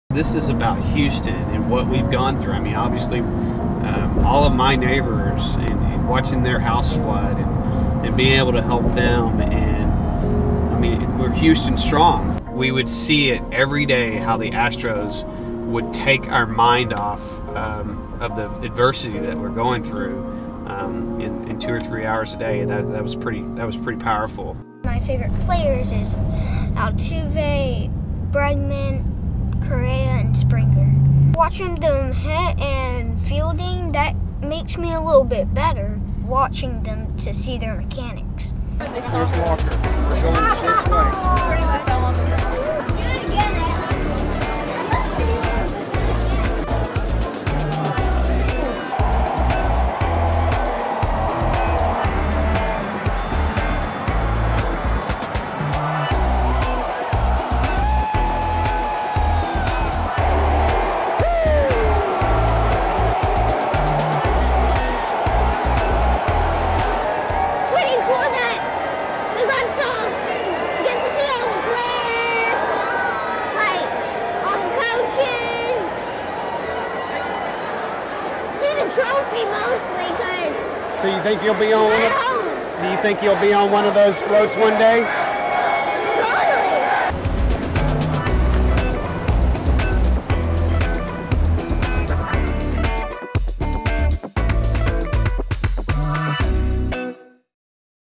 Experience Houston Astros Championship Parade
Follow a father and son as they experience the Houston Astros World Series Parade.